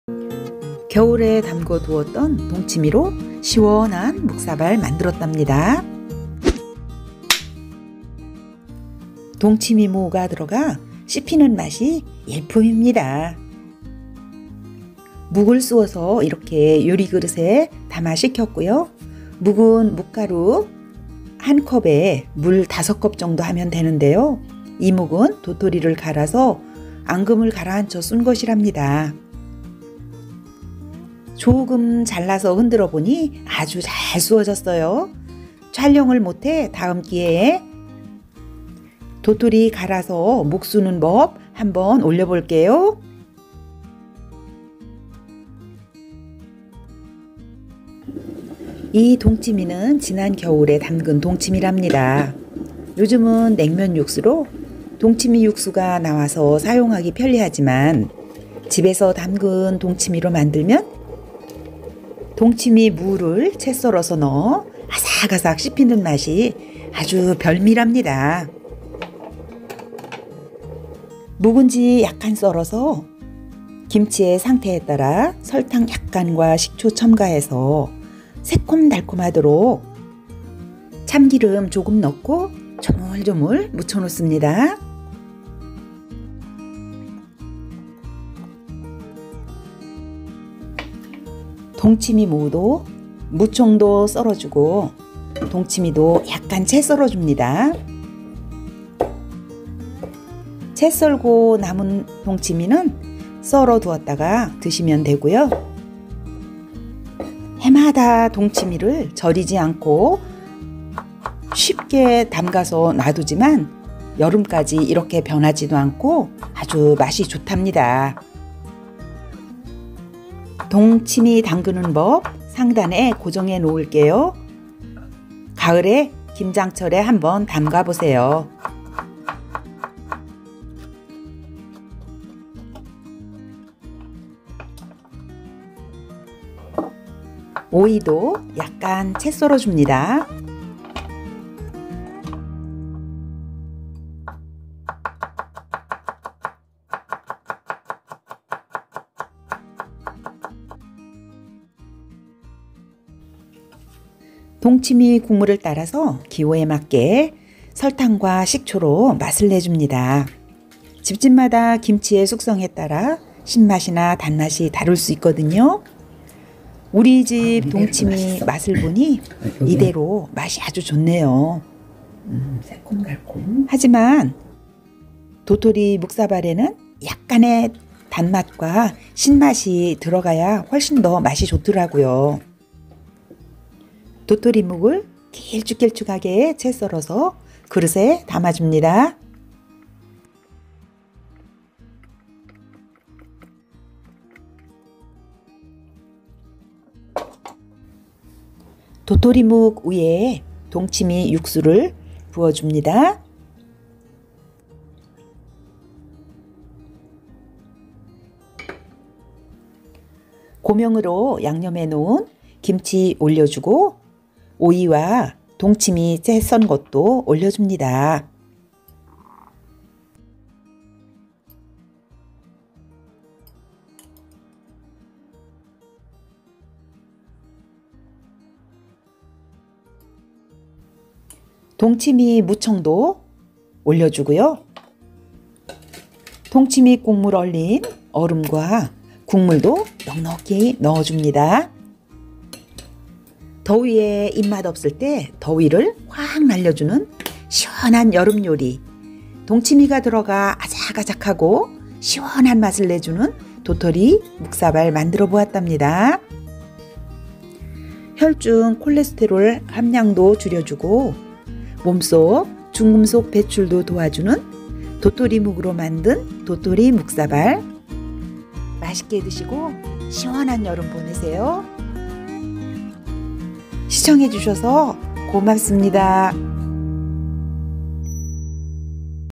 시원한 도토리 동치미 묵사발 만들어 더위를 묵사발 내었어요
겨울에 담그었던 동치미로 묵사발 만들어  더위를 확 묵사발 만들었답니다